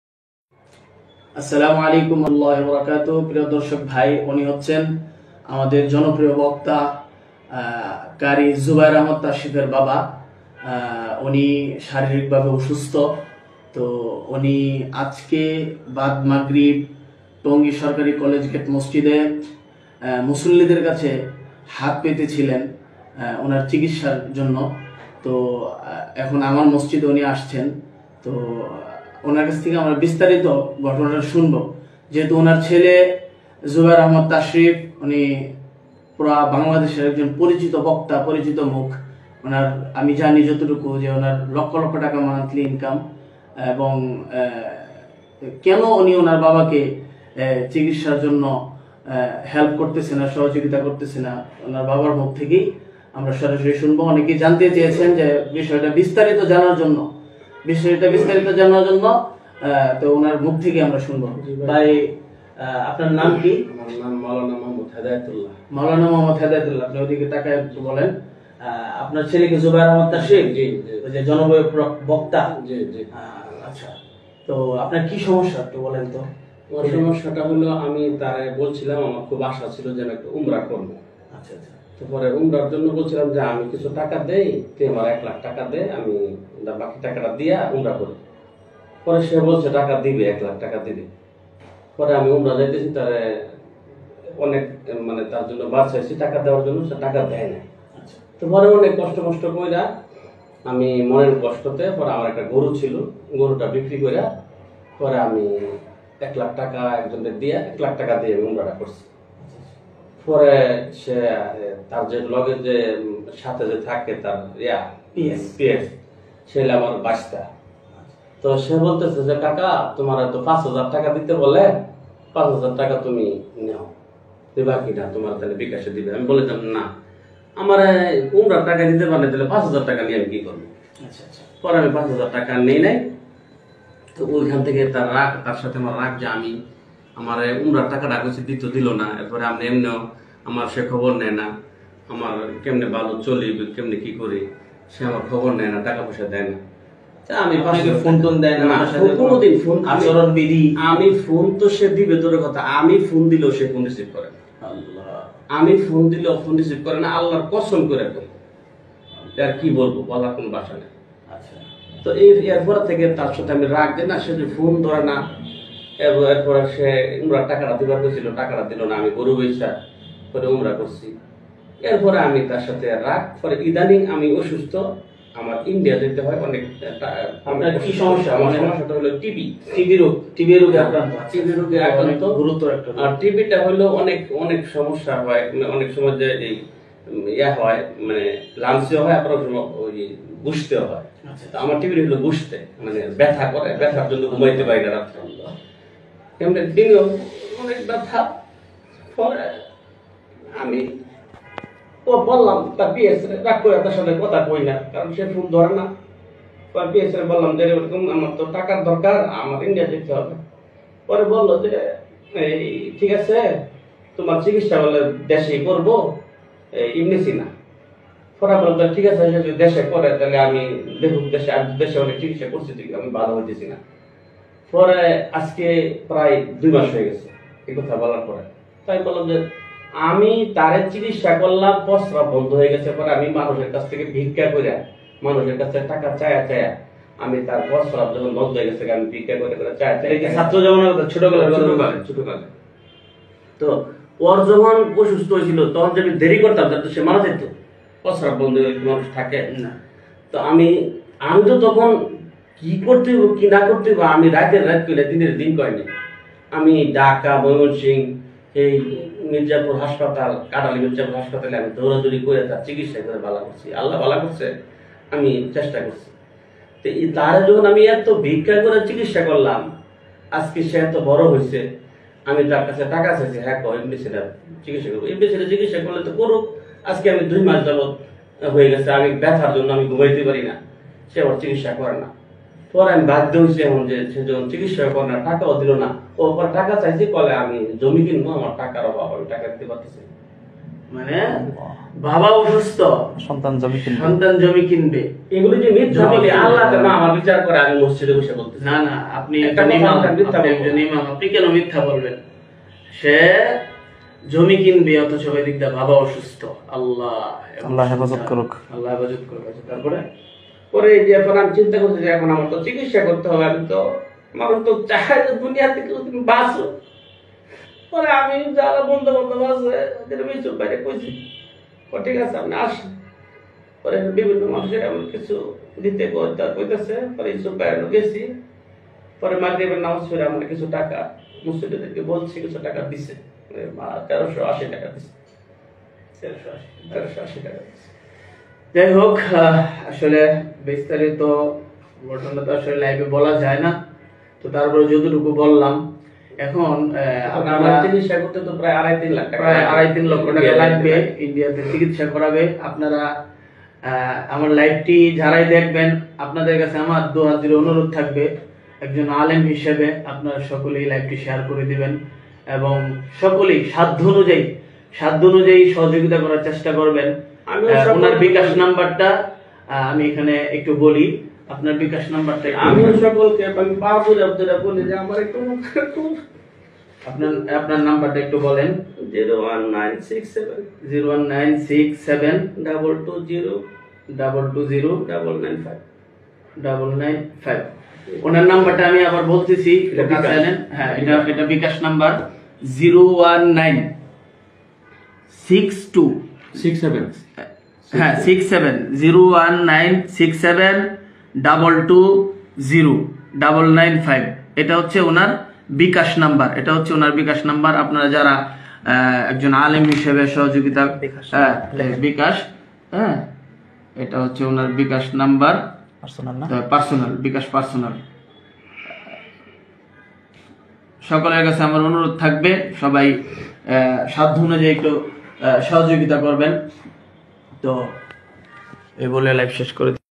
বক্তা নামে কলঙ্ক জুবায়ের আহমেদ তাশরীফ - তার বাবা ছেলের সম্পর্কে কি বলে শুনুন ! Jubayer Ahmed Tasrif
জুবায়ের আহমেদ তাসরিফ বক্তা নামে কলঙ্ক জুবায়ের আহমেদ তাসরিফ বাবার মুখ থেকে শুনেন কি বলে😭 (এই সব সন্তানের জন্য লানত)সবাই ভিডিওটি শেয়ার করে দেখার সুযোগ করে দেন